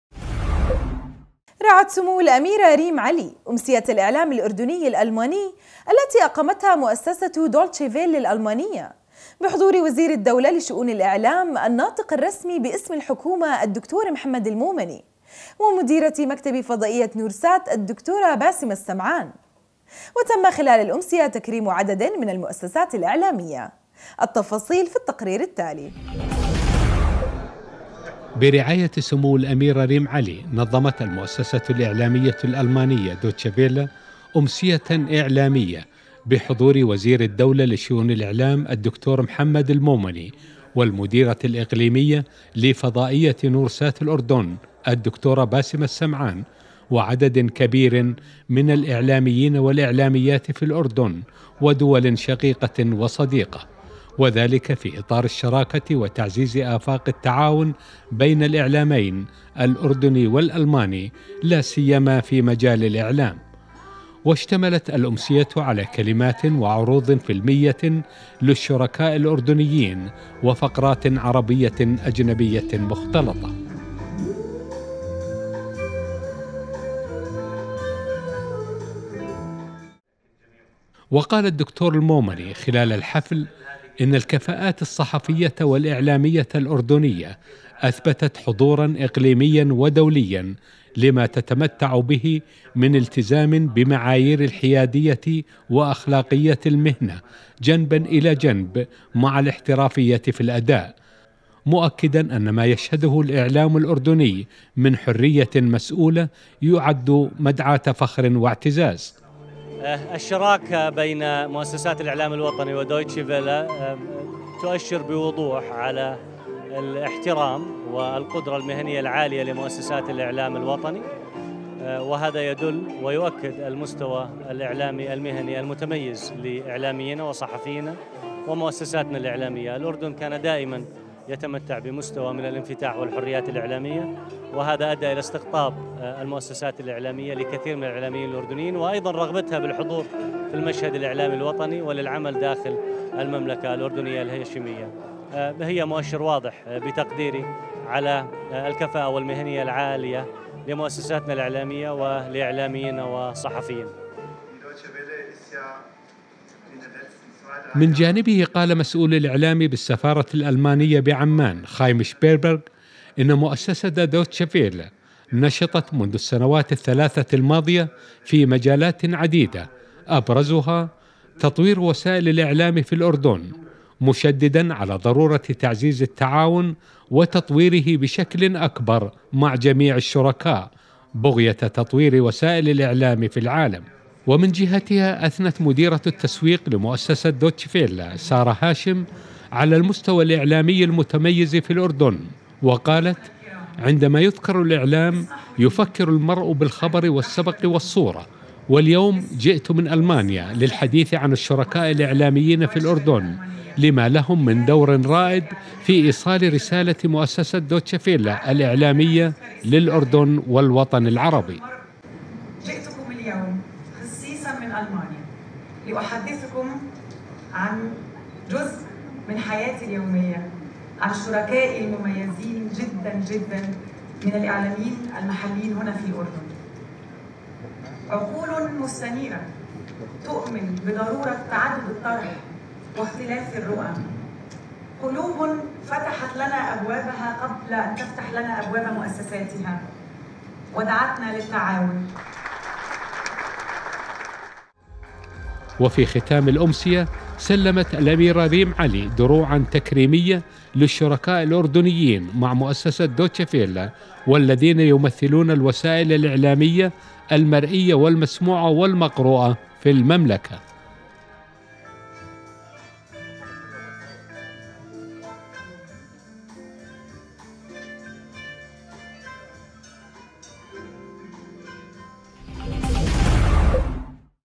نورسات الأردن : تقرير- دويتشة فيله الألمانية /تُكرمُ عدداً من المؤسساتِ الأعلاميةِ الأردنية 2018
رعت سموُّ الأميرة ريم علي/ أمسيةَ الإعلامِ الأردنيِ الألماني / التي أقامتها مؤسّسةُ دويتشه فيله الألمانيّة /بحضورِ وزيرِ الدّولةِ لشؤونِ الإعلام /الناطقِ /الرسمي باسمِ الحكومة الدّكتور محمّد المومني /ومديرةِ مكتبِ فضائيةِ نورسات الاردن /الدكتورة باسمه السمعان ،/ وتمَّ خلالَ الأمسية /تكريمُ عددٍ من المؤسّساتِ الإعلاميّة /... التفاصيل في هذا التقرير